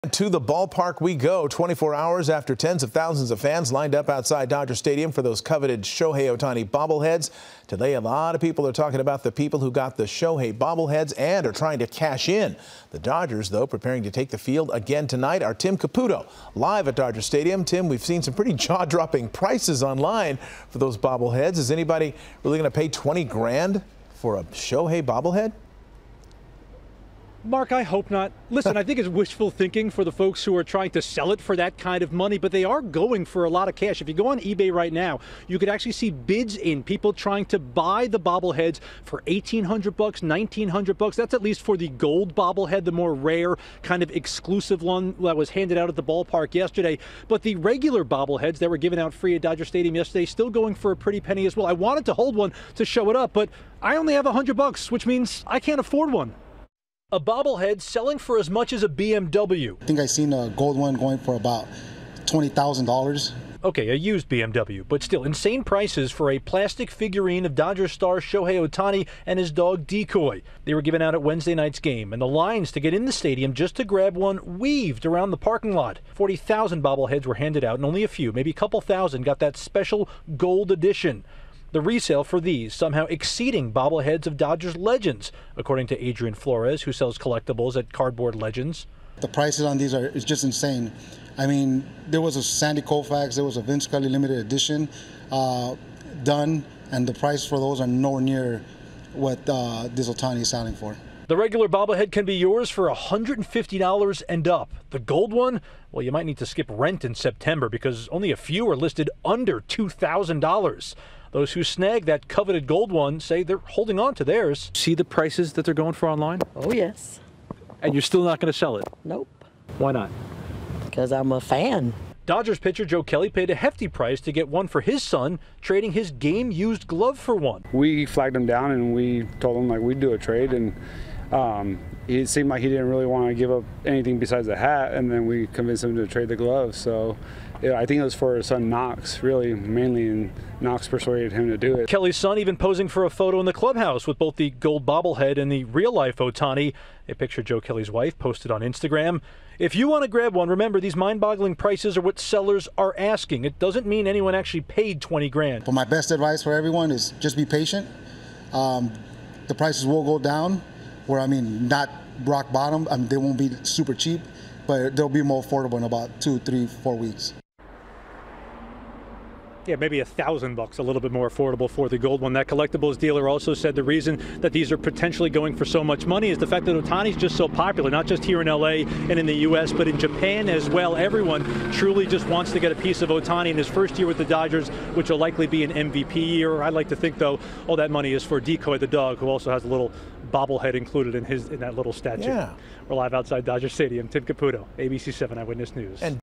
Gold Shohei Ohtani bobblehead selling for up to $20K on eBay
MLB and Dodgers star Shohei Ohtani's dog threw out the first pitch at the game this week, when their bobblehead was given away.